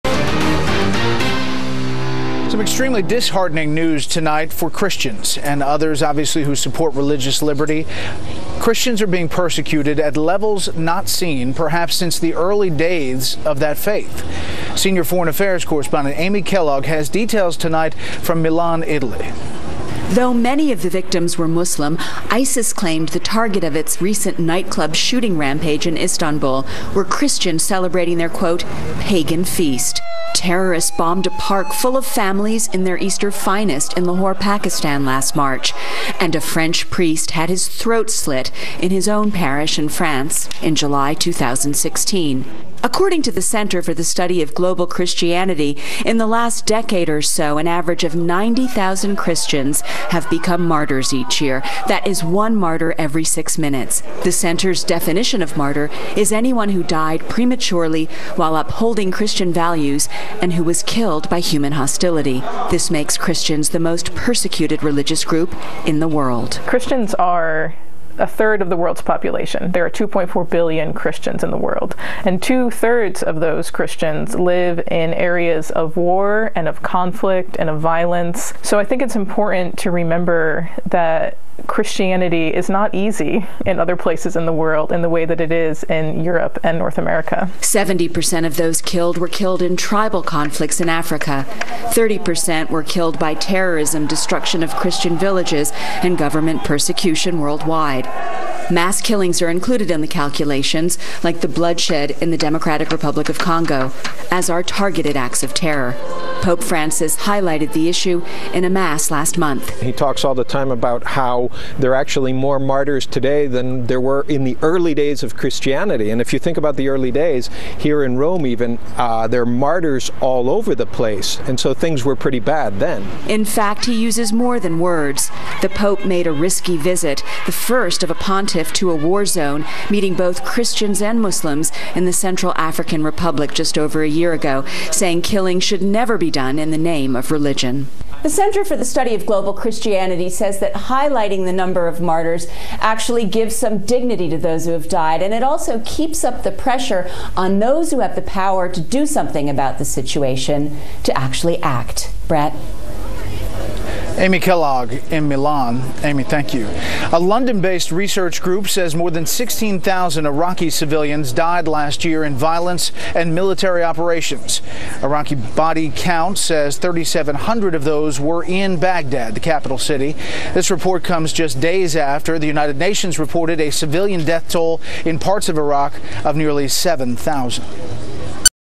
Christians Murdered in Record Numbers by Muslims
By slaughtering Christians, Muslims and those ignoring these murders are publicly admitting that "Allah is Dead" and they must kill people, threaten people and convince people to be muslim with the fear of being murdered because their god allah is an evil spirit of death, darkness, depression, fear, murder, suicide, perversion, and antichrist.

Meanwhile, the life changing message of the gospel of Jesus Christ and Yahweh God of the Bible is actively drawing the world to Himself through the Jesus Christ and God's Holy Spirit.  The god of the Bible is Real and the god of islam is fake.  The false god of Muslims drives them to murder and maim like satan because their founder was Muhammed was possessed by satan.  There is no salvation nor peace in Islam.  Only Jesus Christ has the power of salvation through His life, death and resurrection.